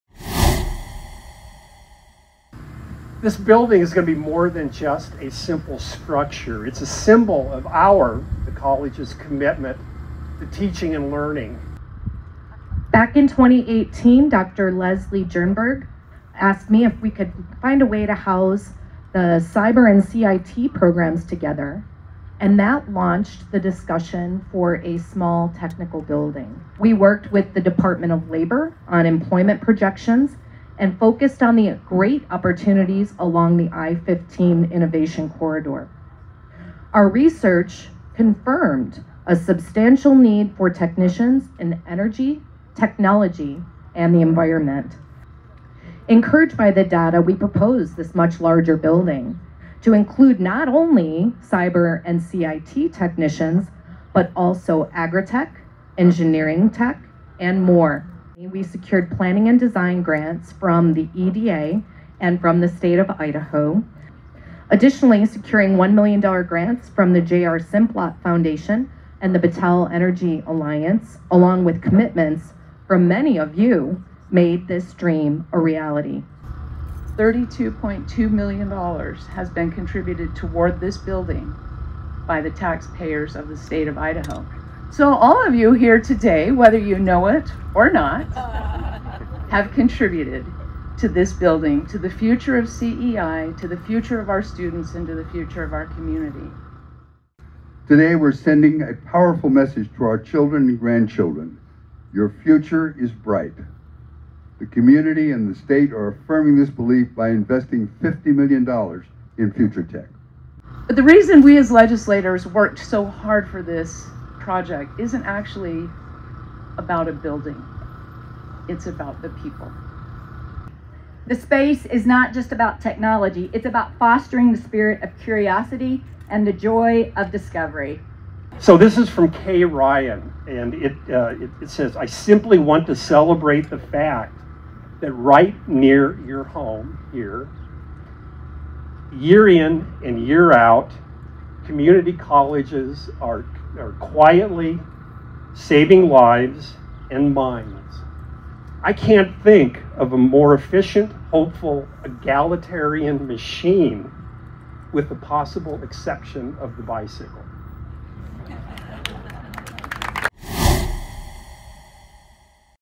It’s official! Construction on CEI’s new Future Tech building begins
IDAHO FALLS – College of Eastern Idaho officially broke ground on its new Future Tech Building Thursday morning.

Construction on the 88,000-square-foot building is now underway in an empty field on the north side of the Idaho Falls campus. The two-level building will house energy, innovation and technology-related courses, such as cybersecurity and mechatronics, as well as IT and STEM programs.

“It’s constructed in a very open way. We don’t know what the careers are going to be in the future and it will be built in a way to accommodate that,” former CEI President Rick Aman tells EastIdahoNews.com.

In addition to classroom space, the building will have a conference center, field testing laboratories and a large gathering area for business and civic events, student group projects and bay space.

The total cost of the project is about $50 million. Of that amount, about $32 million is being funded by taxpayers. The rest of the money is coming from grants and donations from the private sector.

The building is slated for completion in the fall of 2026.